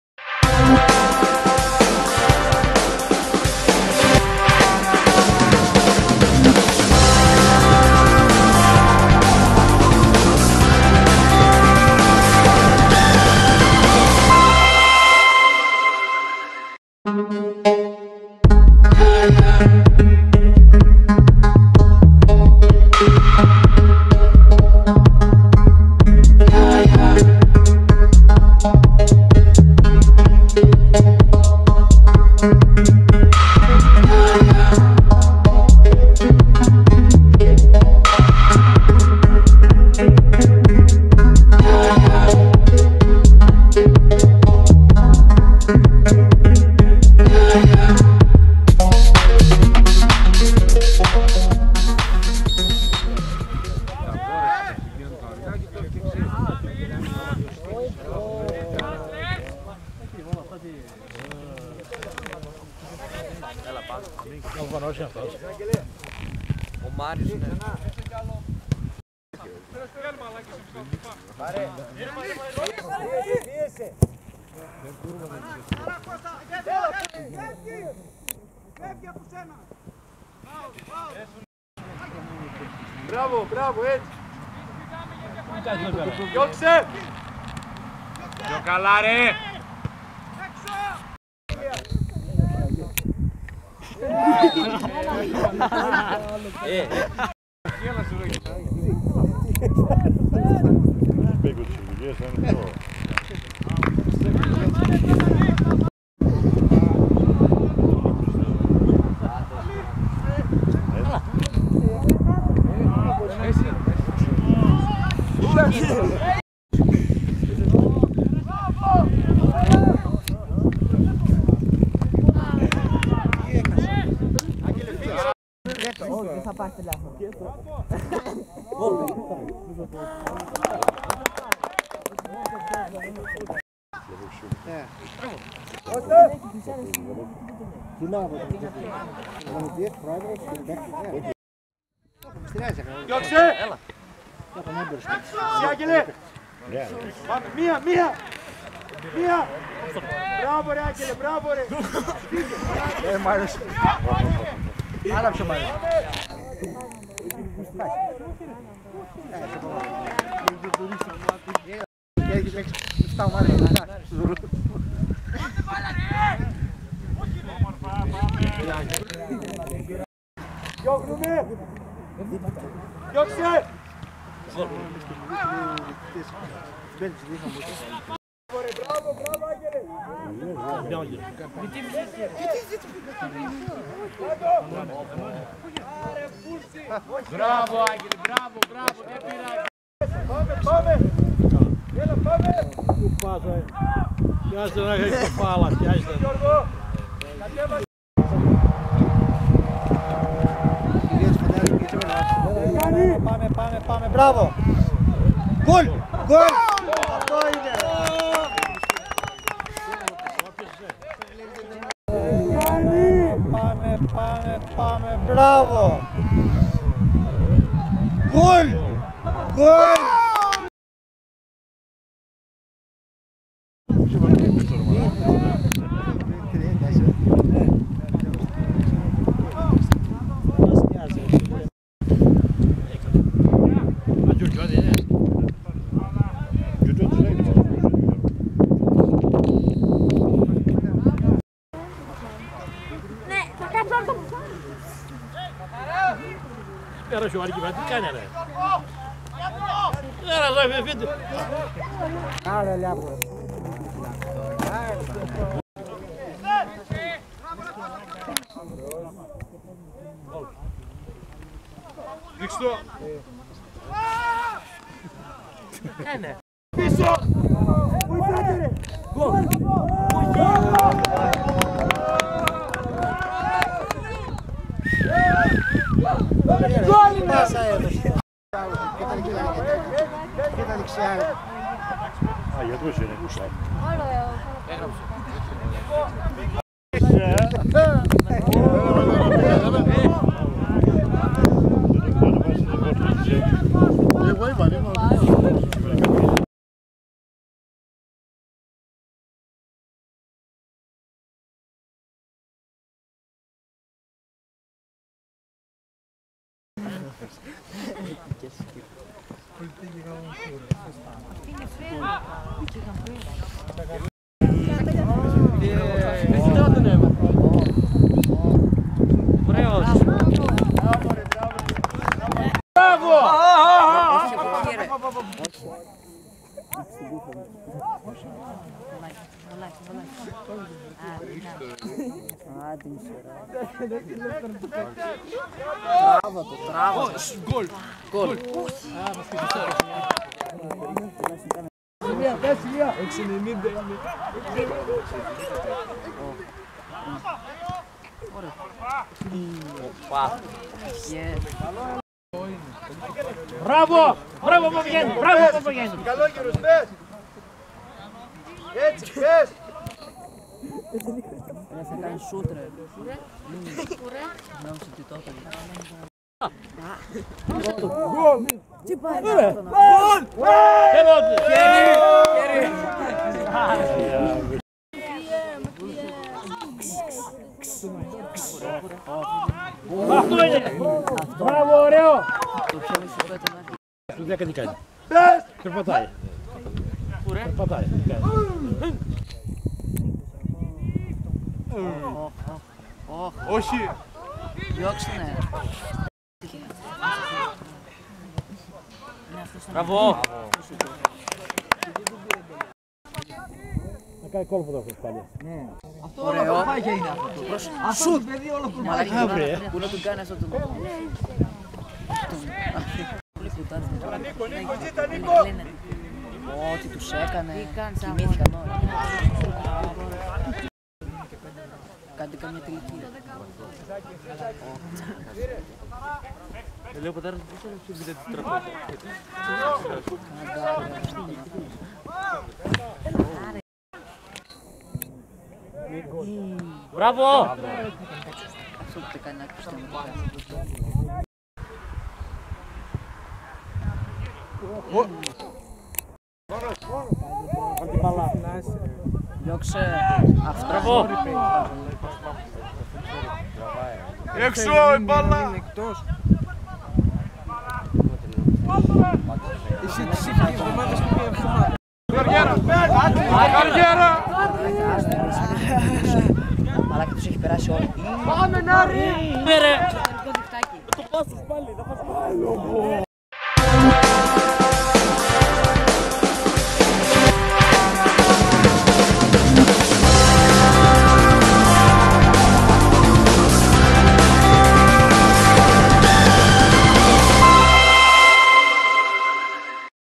Παλαιομονάστηρο - Ροπωτό: 1-0. 5/12/ 2021
6 αγωνιστική για το πρωτάθλημα της Β' ΕΠΣΤ στον β' όμιλο